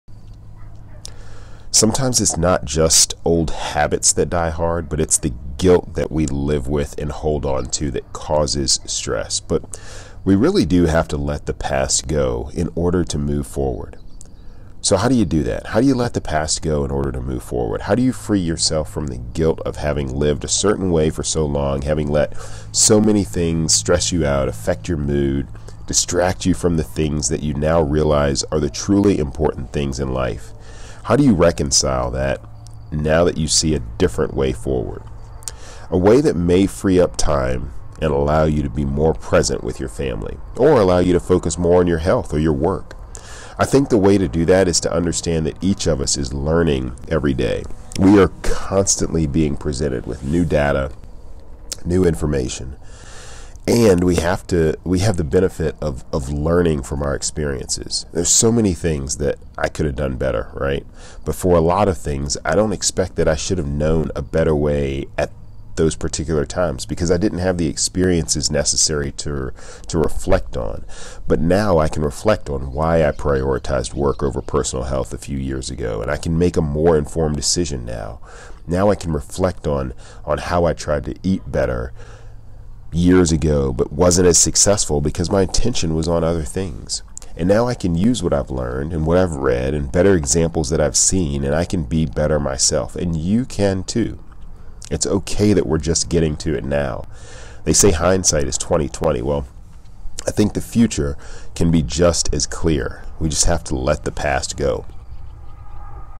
New Information, A New Outlook
How do you let the past go in your attempt to move forward? How do you free yourself from the guilt of having lived a certain way for so long, having let so many things stress you out, affect your mood, distract you from the things that you now realize are truly important in life…? How do you reconcile with all of that now that you see a different way forward? The way to do that is to understand that each of us is learning every day. We are constantly being presented with new data and new information, AND we have the benefit of learning from our experiences. They say hindsight is 20/20, but the future can be just as clear. We just have to let the past go.